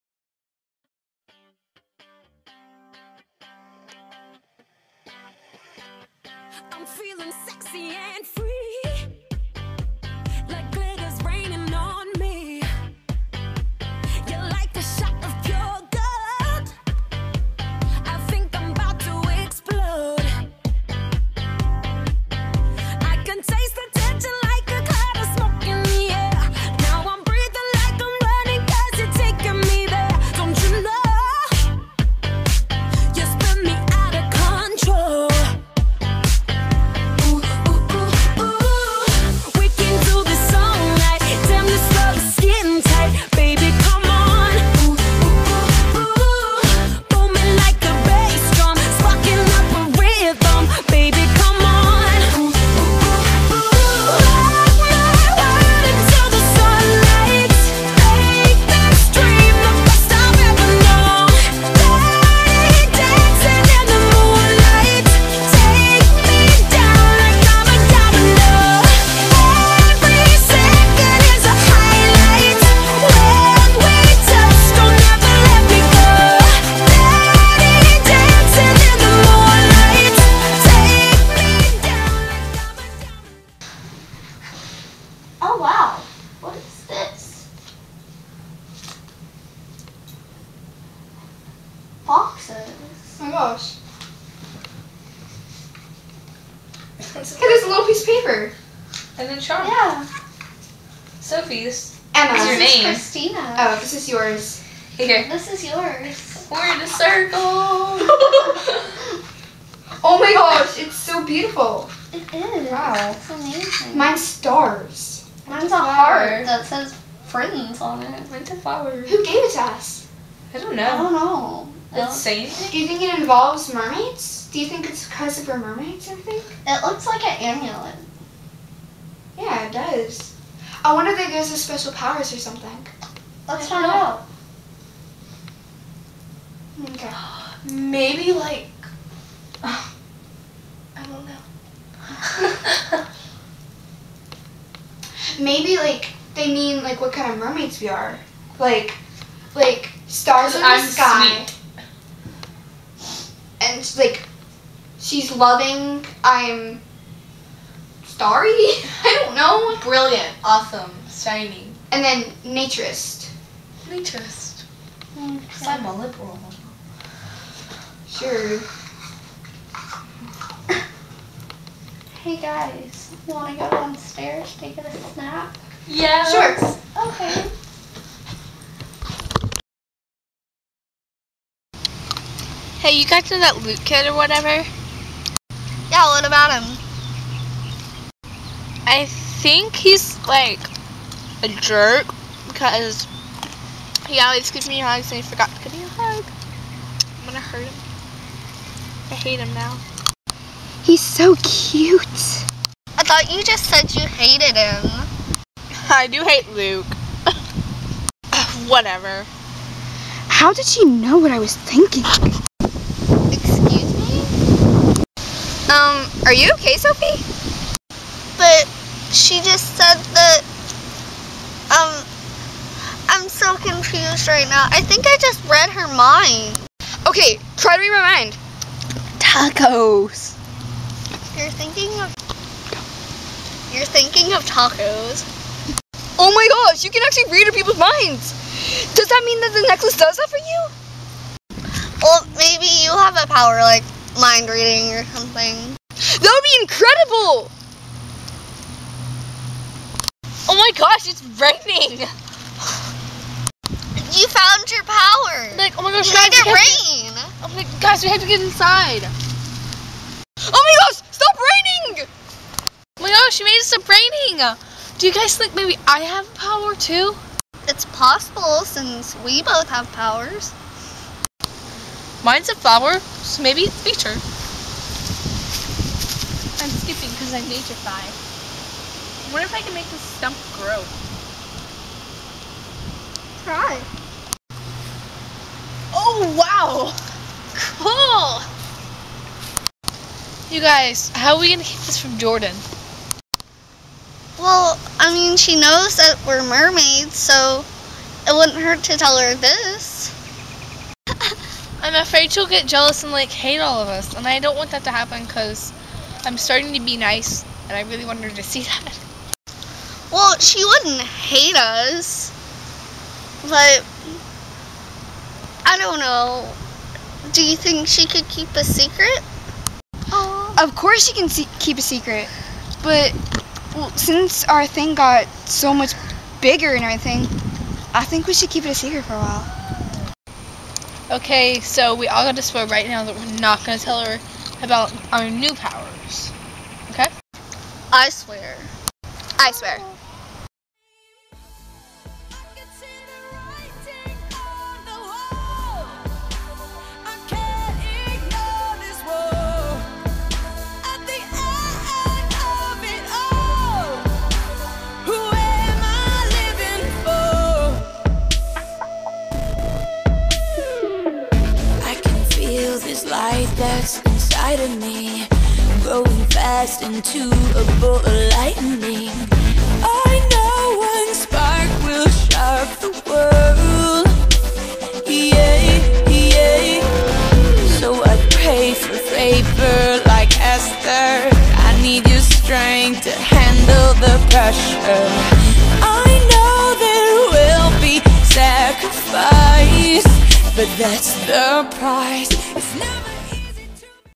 My Best Friend Is a Mermaid Season 2:Episode 1-Promises
tiny boxes were in the big pink box. They had necklaces in them.
Sophie's necklace was a silver heart.
Kristina's necklace was two tiny stars.
Emma's necklace was a gold flower.
When they go outside they find out the necklaces give them powers.
Sophie's power is to read minds.
Kristina's power is to control weather.
Emma's gift is to control nature (plants).
They decide not to tell Jordon about it. What will happen next?